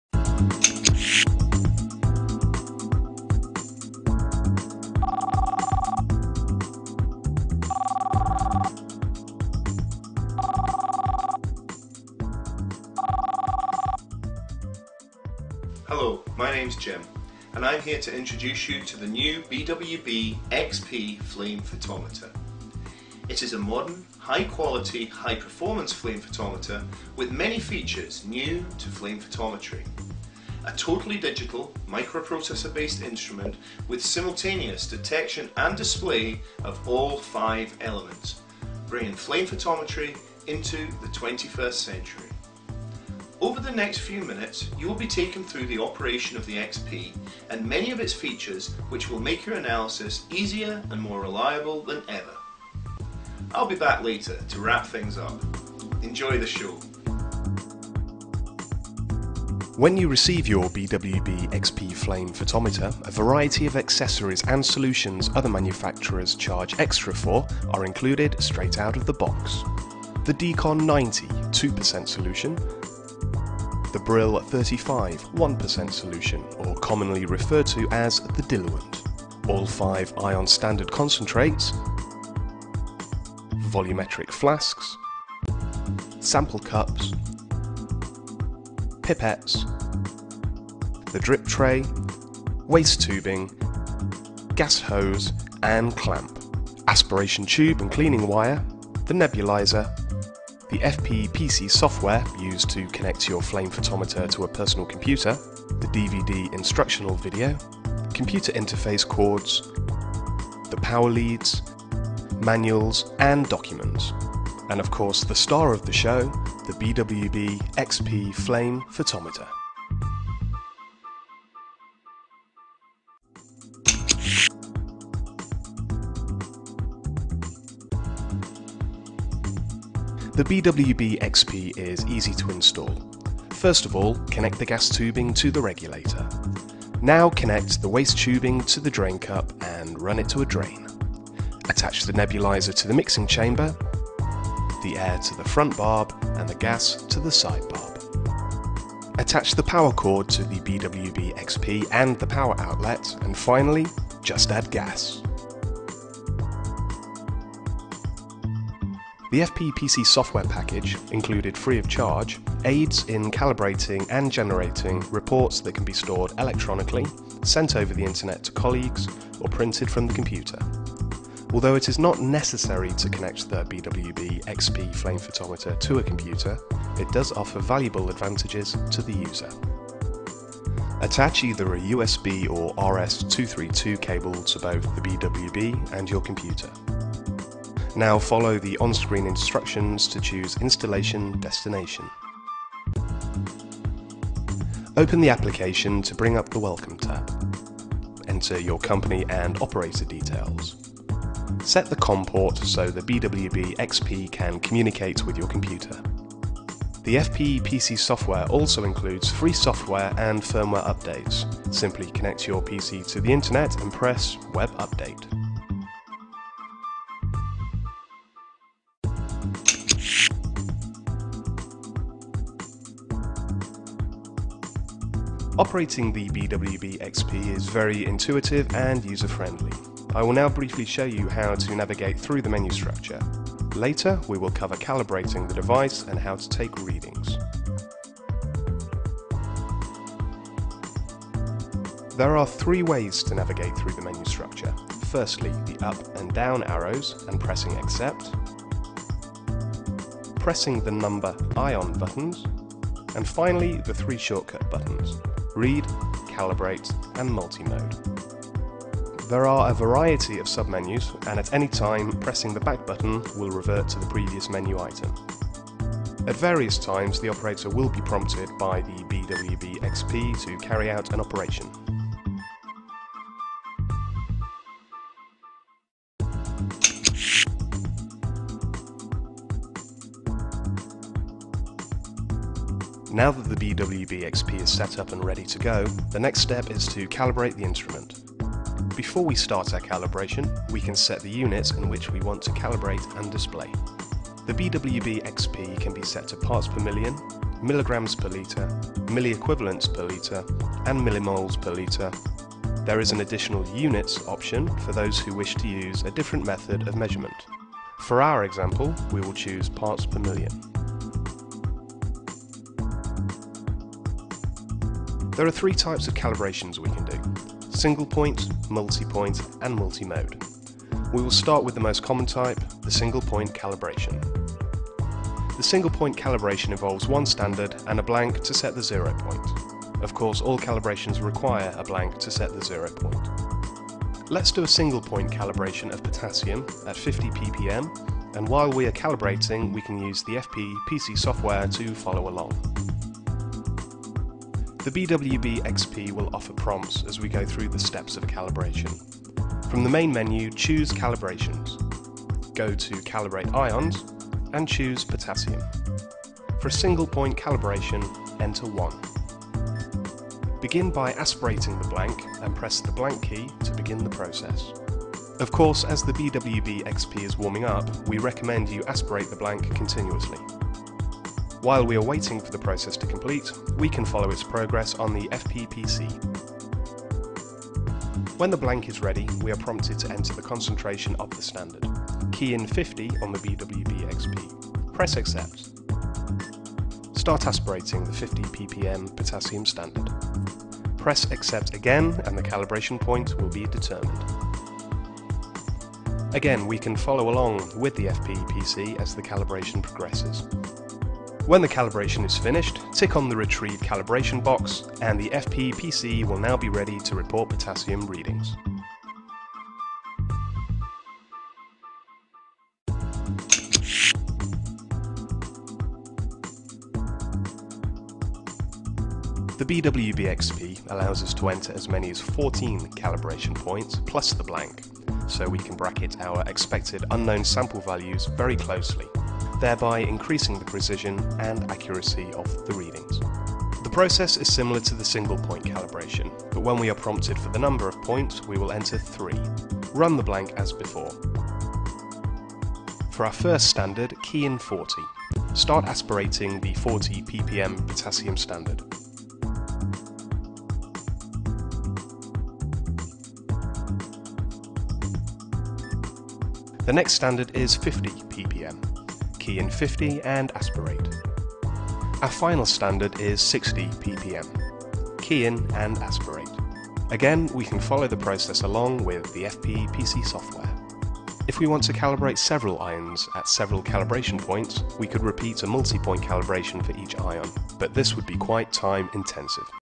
BWB XP Flame Photometer-1
The BWB XP Flame Photometer
Our award winning 5 channel instrument
Na, K, Li, Ca, Ba

The BWB XP is the first and only 5 channel flame photometer
with simultaneous detection and display of all 5 elements.
The BWB XP was designed from the ground up, using new, innovative and modern technology to give unsurpassed levels of accuracy, usability and reliability while significantly reducing analysis time. Our customers do not need to waste valuable time changing filters and re-calibrating when a different measurement is required, as is a common requirement in all other Flame Photometer designs from 1951 to the present.
With clean lines and contemporary bright colour schemes, the XP looks exceptionally modern and will fit into any laboratory. The instrument has a large LCD display, which can be viewed easily from any angle.
Our metal casework provides a sturdy portable instrument and enables future recycling opportunities. ROHS compliant components and materials are used wherever possible.
The BWB Carbon Footprint is exceptionally low, in keeping with corporate objectives of being a responsible manufacturer.
Unlike other manufacturers who charge extra for vital accessories, BWB include everything required to use the product straight out of the box. Just add Gas!! (Propane, LPG or butane).
We also provide as standard, data sharing via USB from the BWB XP to your PC. This ties the output of the BWB Flame Emission Spectrometer (FES) to the Internet or your company intranet systems and facilitates Distributed Control which is vital for Multi-National companies, Universities and various Governmental agencies.